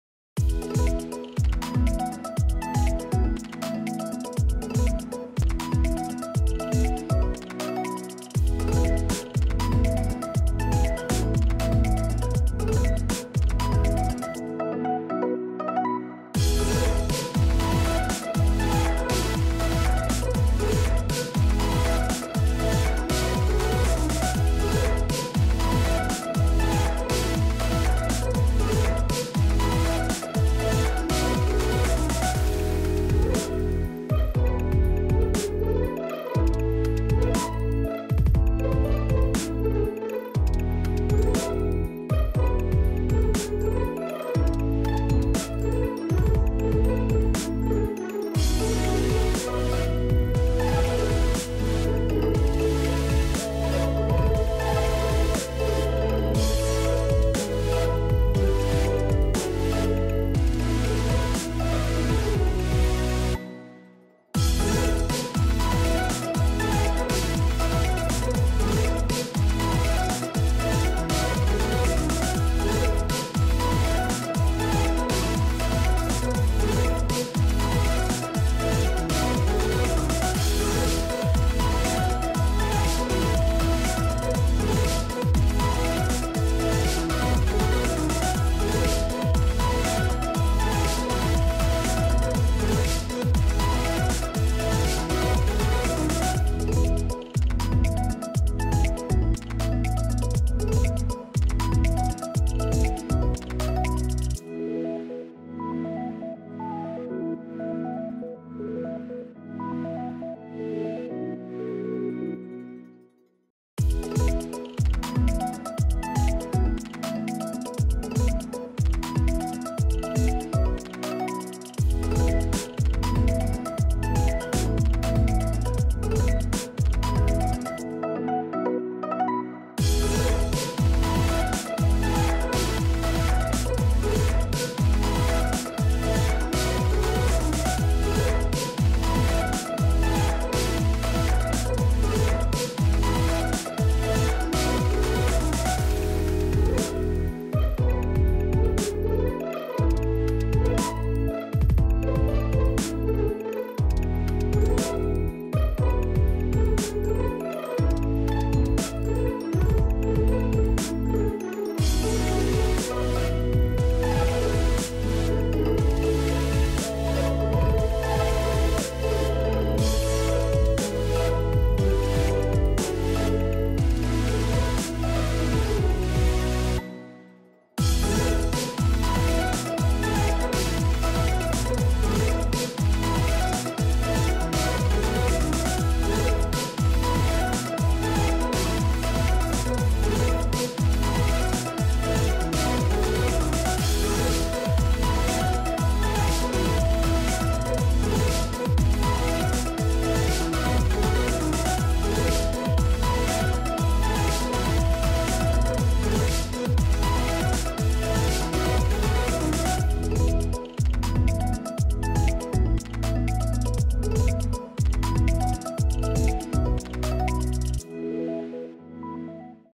【描いてみた】ポケモンフュージョンで生まれたキメラを描く【超倍速⚡】│Drawing monsters made with Pokemon Fusion Generator
配信で描いてたものを早送り編集したやつです📺